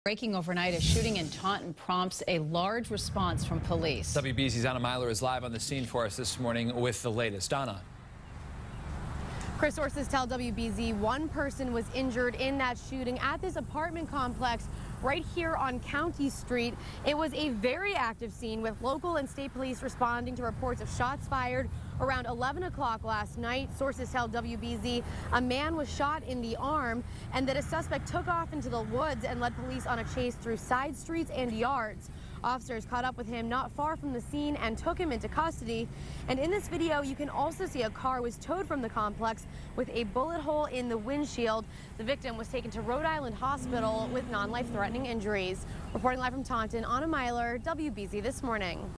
1 Hurt, 1 Arrested In Taunton Apartment Complex Shooting
A shooting at a Taunton apartment complex left one person injured and another under arrest Wednesday night, prompting a large police response. WBZ-TV's Anna Meiler reports.